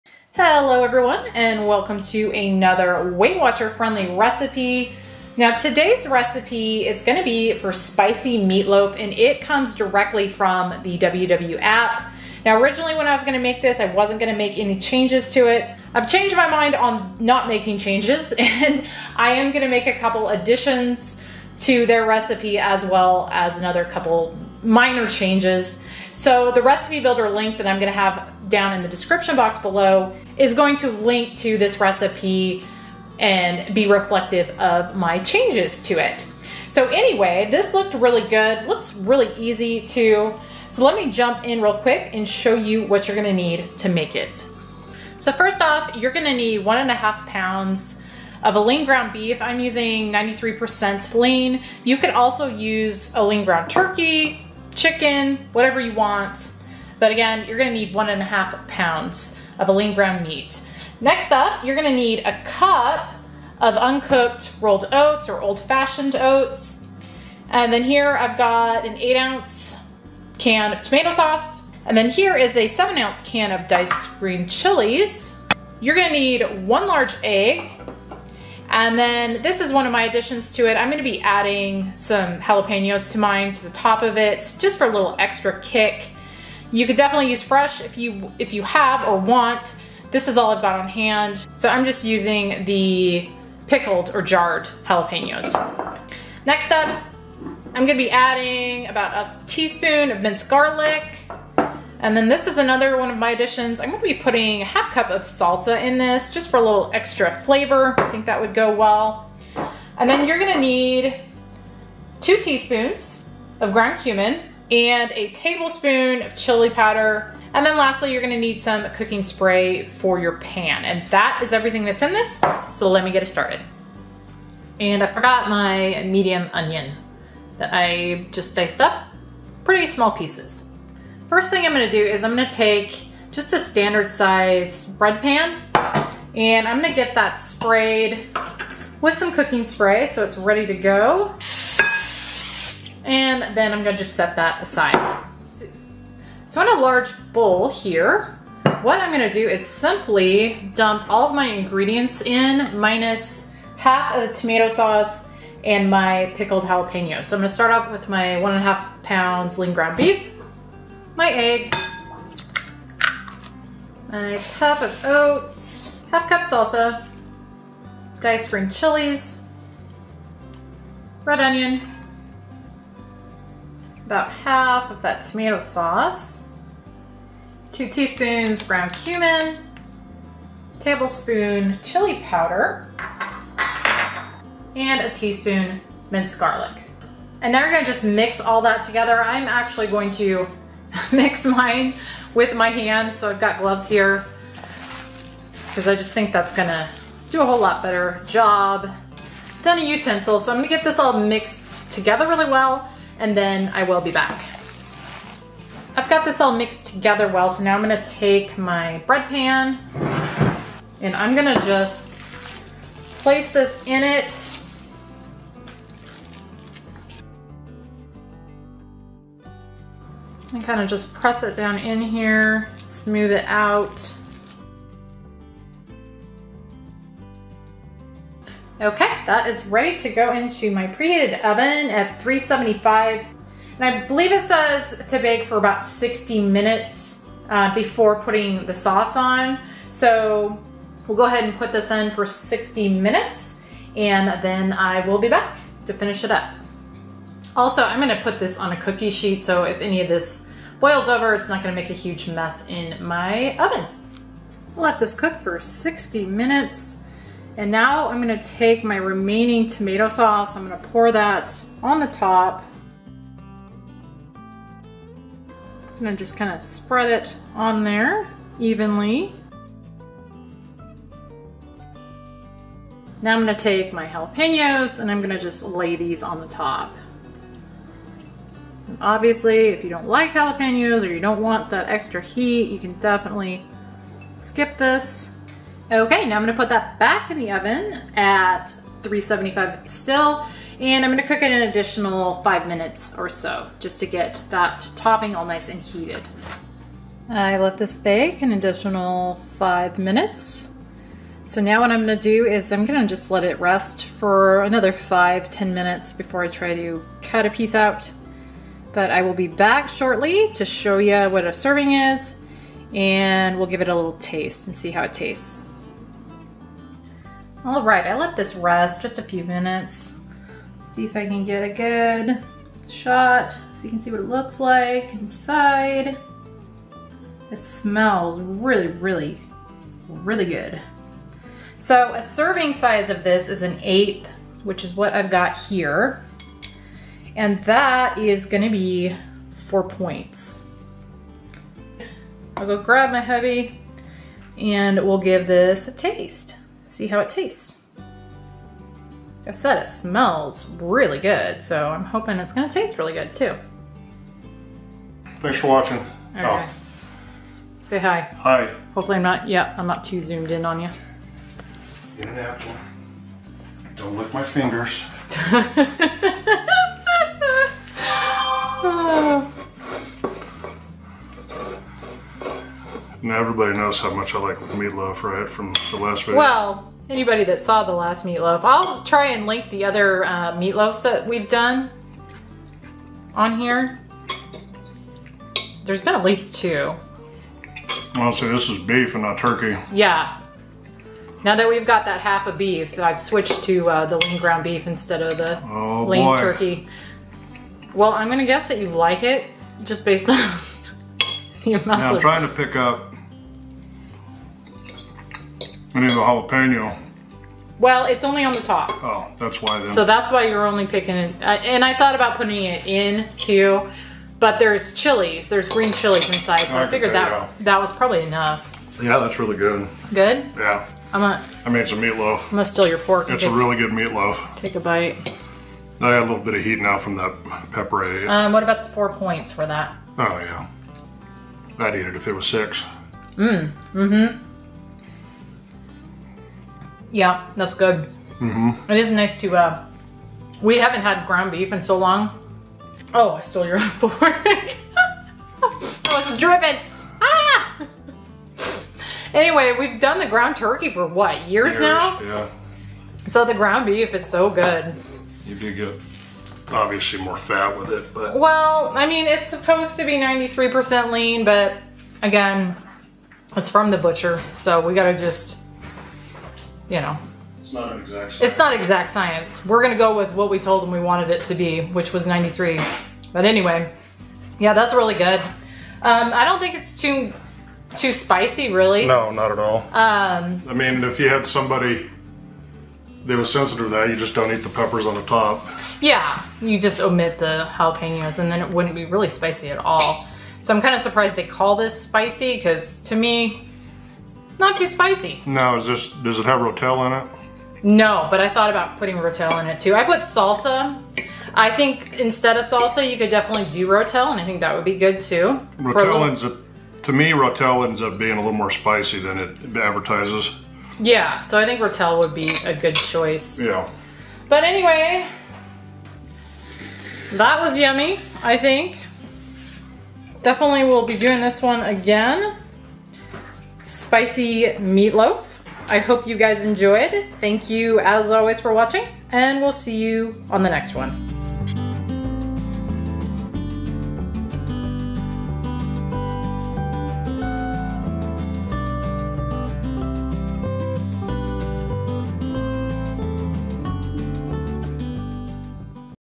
EASY Spicy Meatloaf- Weight Watchers Recipe- WW Dinner Meal Prep- With WW PTS, Calories/Macros!
EASY Spicy Meatloaf- Weight Watchers Recipe- WW Dinner Meal Prep- With WW PTS, Calories/Macros!  I tried out a new recipe this week with this spicy meatloaf. I found the recipe in the WW app but made some changes to it to make it more to our liking! It was delicious was the perfect cozy comfort food for a cool Fall day! Weight Watchers Points & Calories/Macros and additional nutritional information Included down below!

If you join WW following this link, you will receive a FREE month!

Ingredients:
1 1⁄2 pounds Uncooked 93% lean ground beef
1 cup Uncooked rolled oats
1 medium Onion, finely chopped
6 oz, mild or spicy, diced Canned green chile peppers
1 large egg
1 Tbsp Chili powder
2 tsp Ground cumin
8 oz Canned tomato sauce, divided
1 tsp minced garlic
1⁄2 cup Fat free salsa
1 oz Pickled jalapeno peppers
 Cooking spray
Spray a standard size loaf pan with cooking spray. Combine all ingredients, except half of tomato sauce, in a large mixing bowl. Place meatloaf mixture into prepared pan, pressing in and smoothing the top. Bake at 375°F for 60 minutes. Spread remaining sauce over meatloaf and jalapenos, and cook 5 minutes more.
Makes 8 servings. WW Points per serving - 4 PTs.  If you are a WW member click this link to take you to the recipe and Recipe builder➡

*Nutrition Information: YIELD: 8 SERVING SIZE: 1/8
Amount Per Serving: CALORIES: 208 TOTAL FAT: 7 g SATURATED FAT: 3 g CHOLESTEROL: 76 mg SODIUM: 598 mg CARBOHYDRATES: 14 g FIBER: 2 g SUGAR: 4 g PROTEIN: 20 g
*Nutritional Information was calculated using myitnesspal, values may vary based off ingredients selected.

Some of my favorite Kitchen things!!